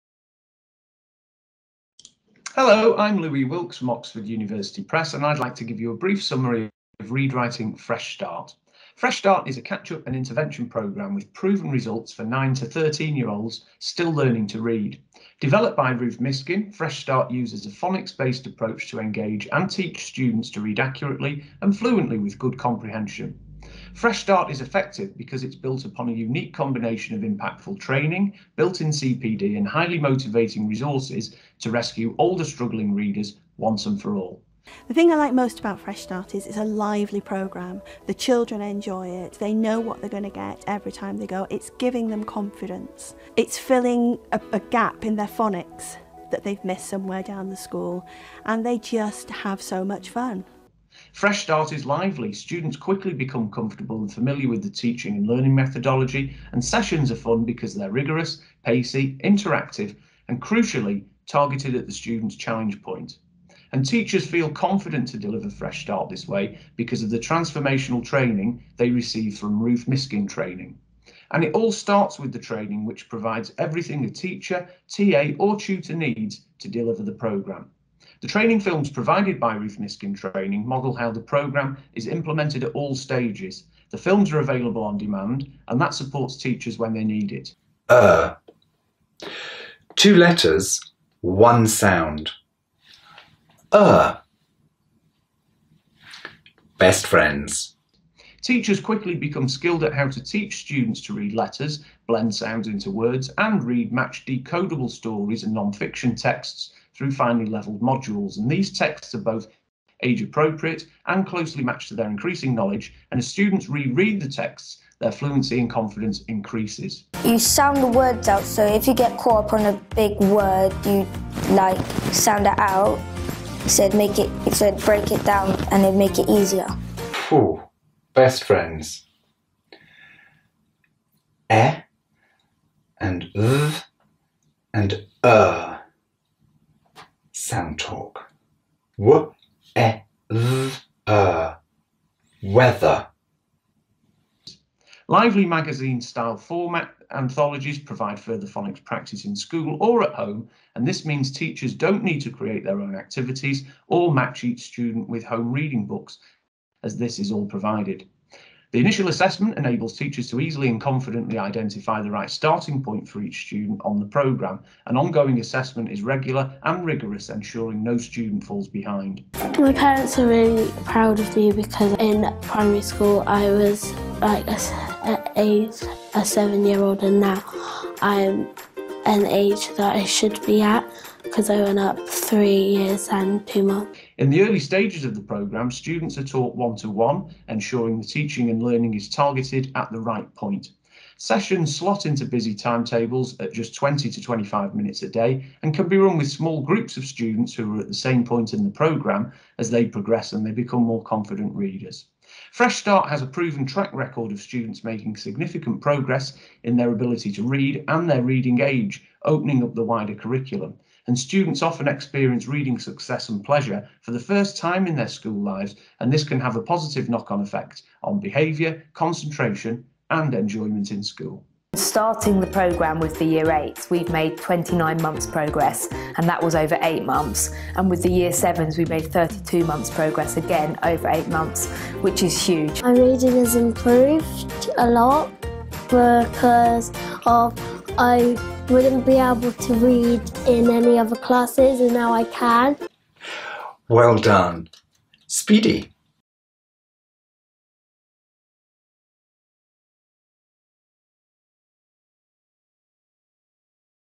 A quick introduction to Read Write Inc. Fresh Start
Hear from Louis Wilkes at Oxford University Press about Read Write Inc. Fresh Start, a catch-up and intervention programme for children aged 9 to 13 who are still learning to read. Teachers and students using Fresh Start also share their experiences of the programme.

Find out more about the training for Fresh Start from Ruth Miskin Training